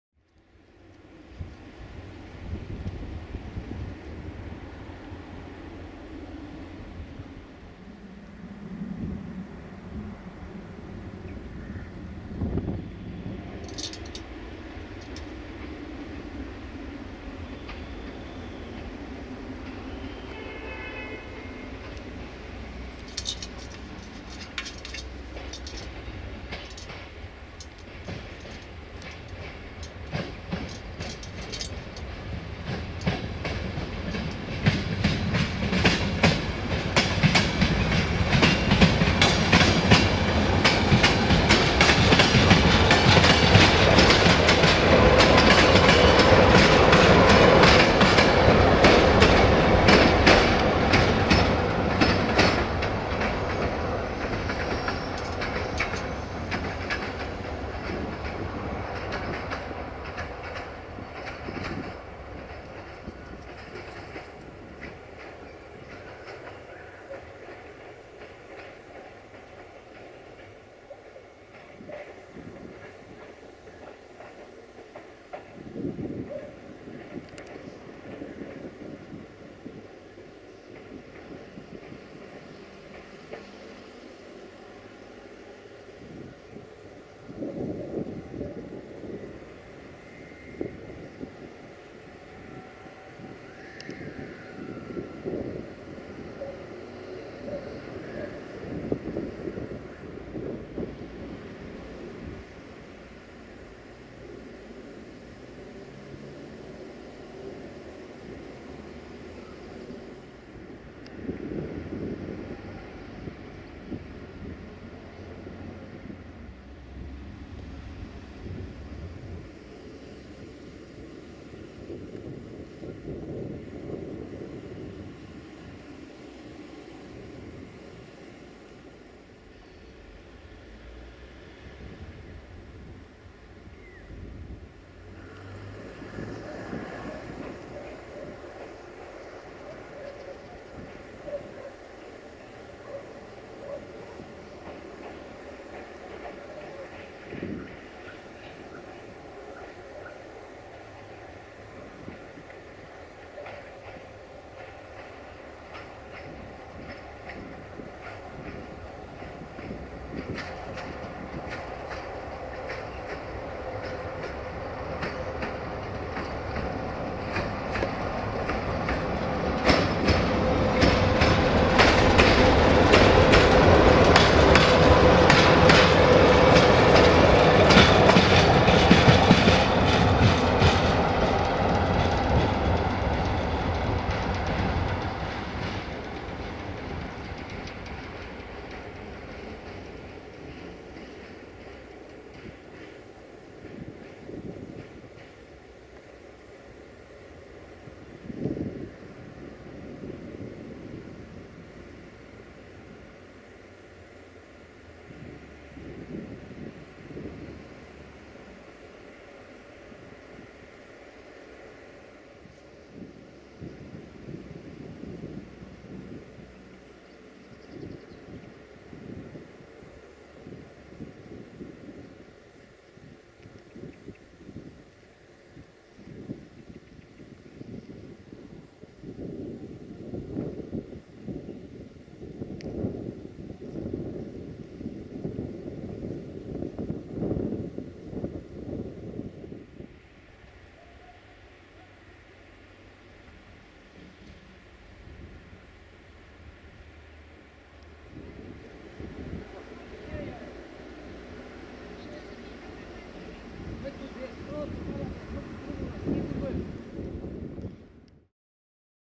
LDH 89-0309-1 la Manevra/Shunting in Gara Simeria Station pe Apus/on Sunset - 21 April 2017
1:Manevra/Shunting - LDH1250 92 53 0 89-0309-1 RO-SNTFC

21 April 2017